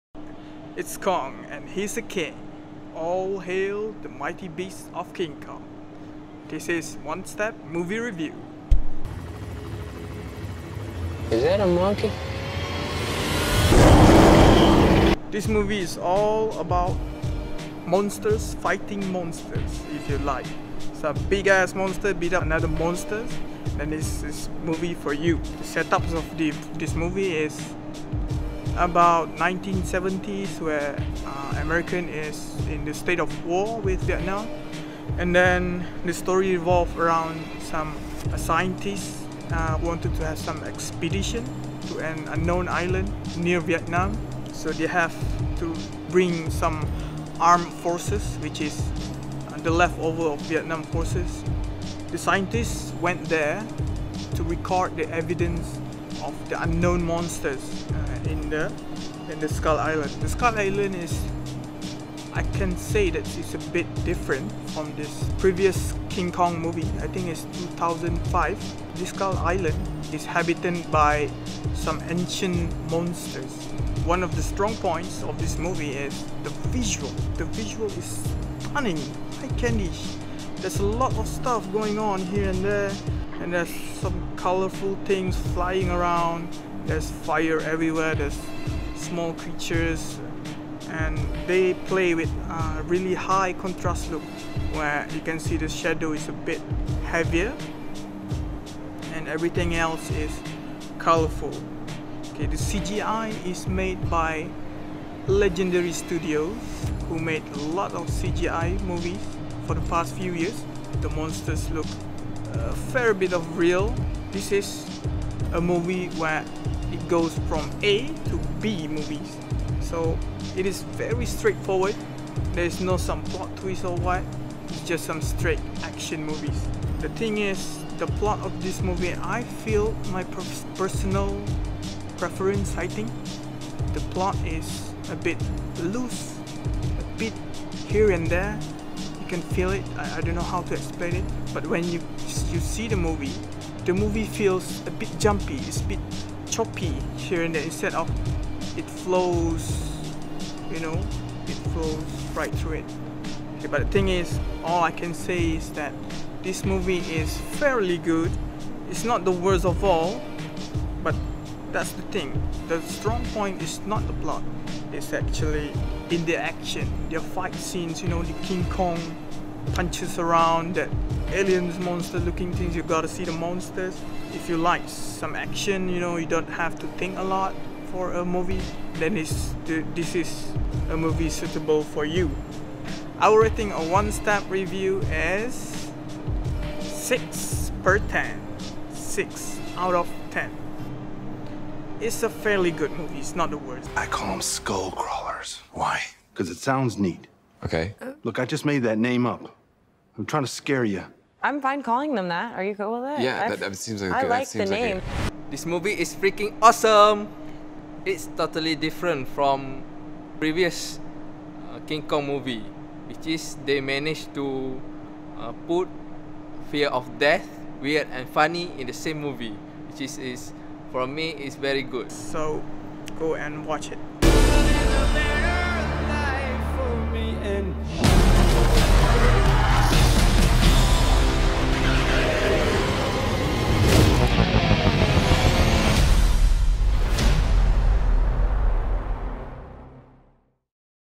Oversized King Kong | Movie Review
We went to see an oversized monkey in action!
Here's our thought about it.

If you liked the video, smash that subs button!